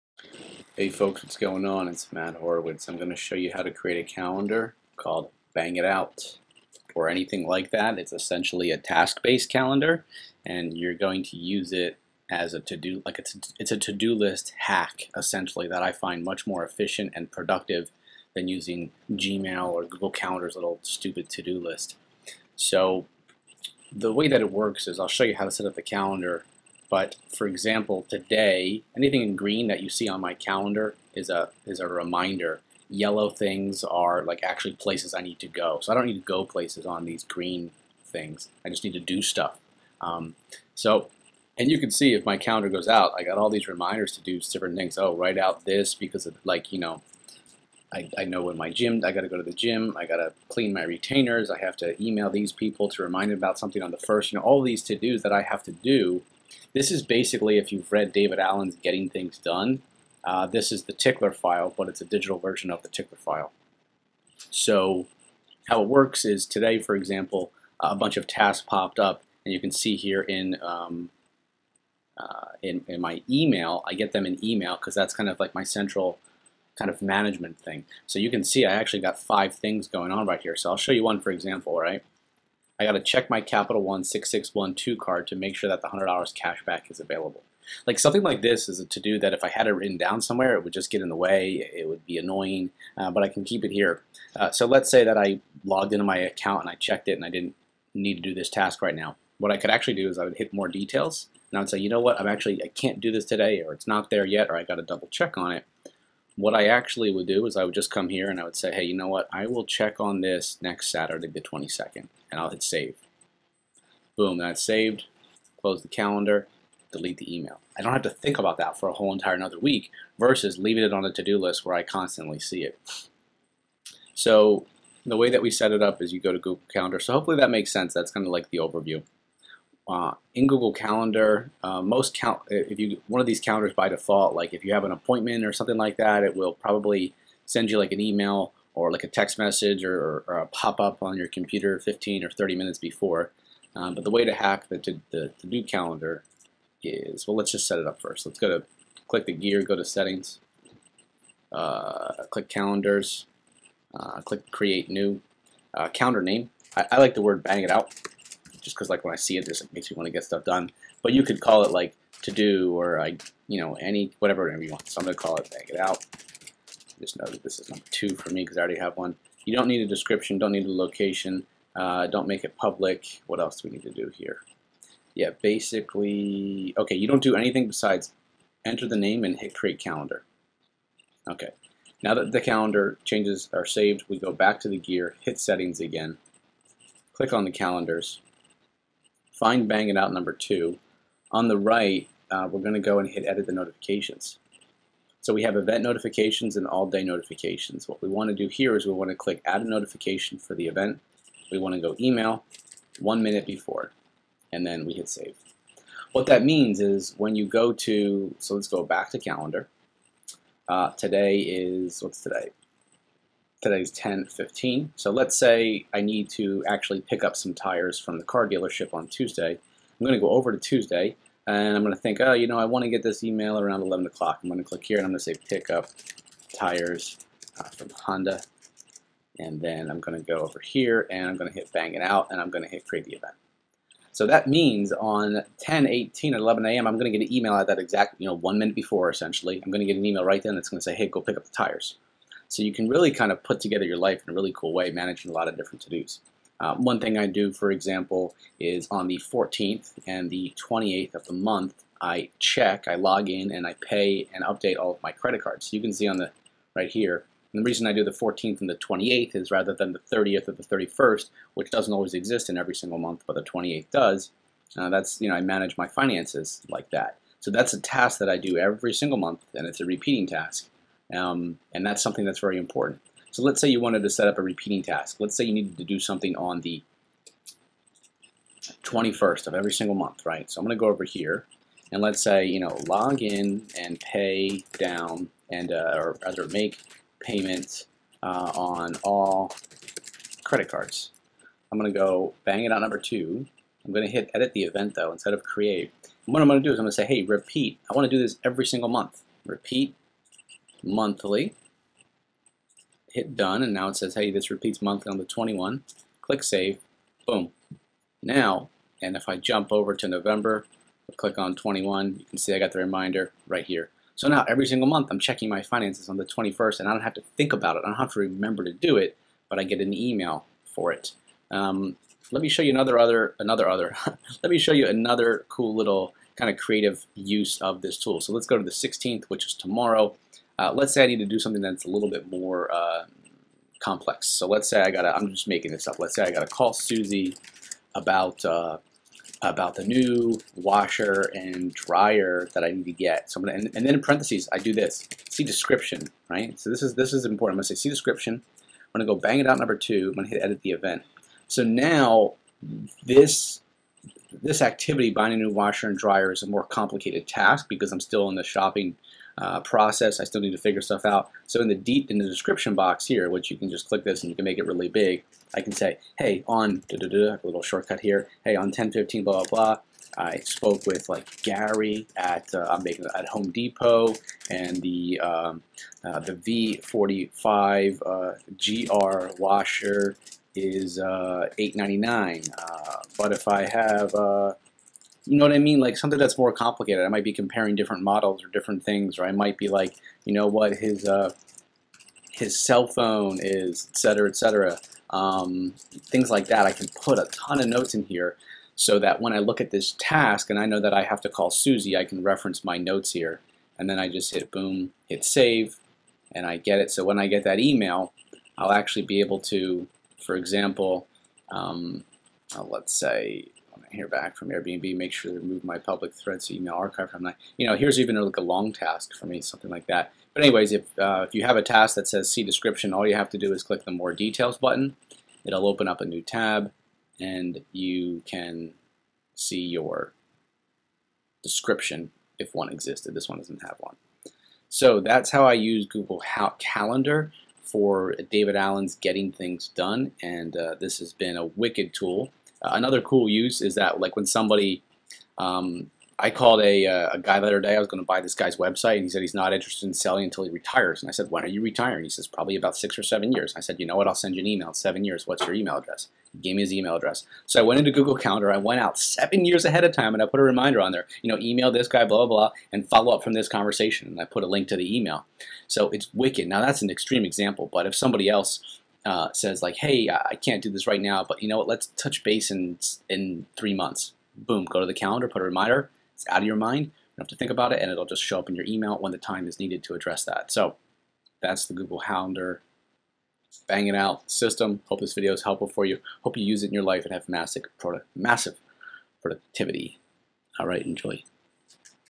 Use Google Calendar for Getting Things Done (David Allen's Tickler File)
Here is a "hack" I've been using with Google Calendar for years. This will help with getting things done, setting repeating reminders tasks, and overall, just staying on top of life and crushing it.

Let me know if you have any questions. Enjoy!